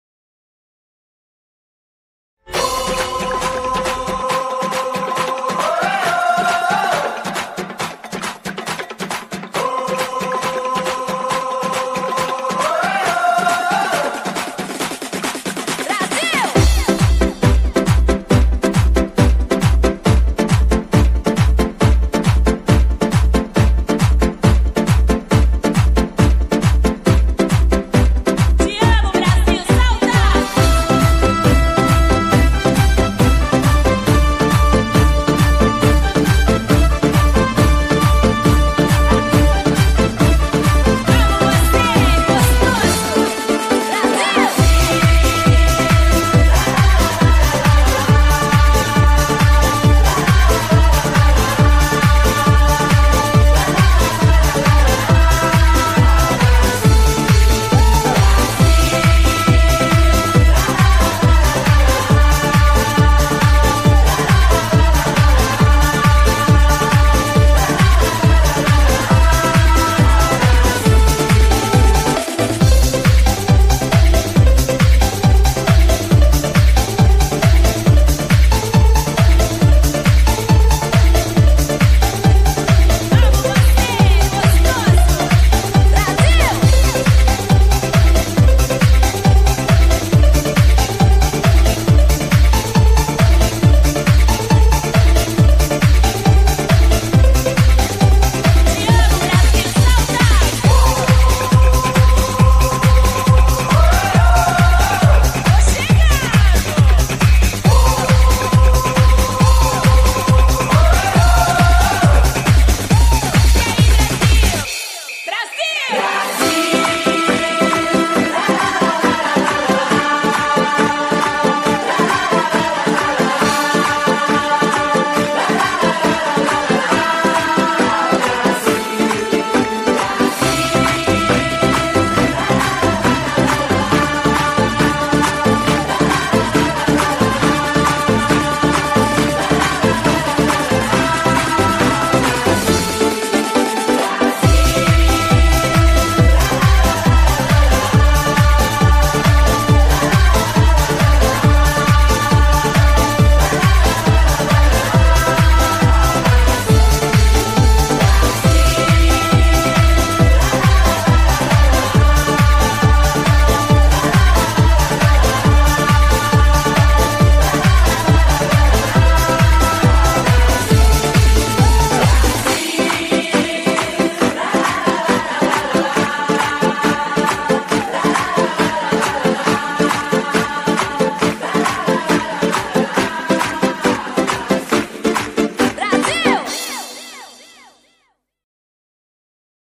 Aerobic Dance with dumbbells| Cristmas & New Year Celebration
Aerobic dance with dumbbells on cristmas & new year 2023 celebration.